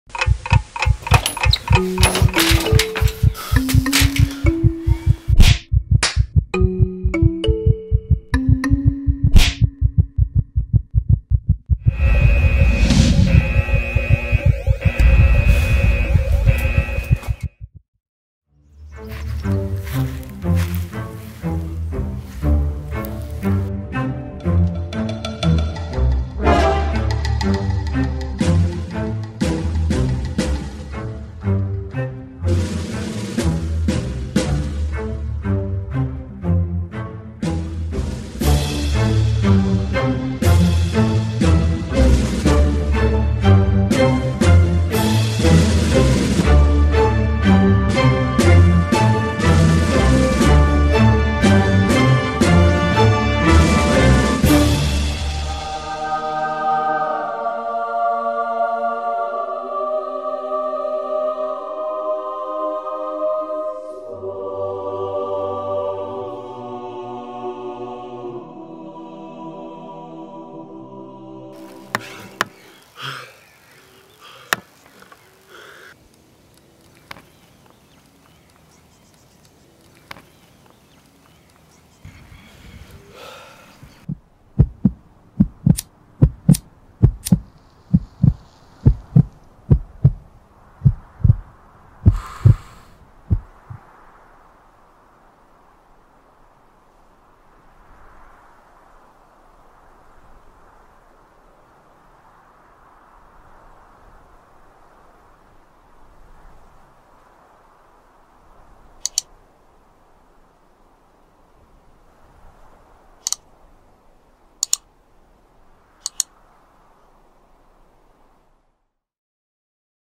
Morning Cigarette | Anti-Smoking Video
This is just something pretty stupid I'll admit. I handed it in as an assignment for college, we had to make an advertisement or psa of some kind and I honestly just wanted to have fun with it.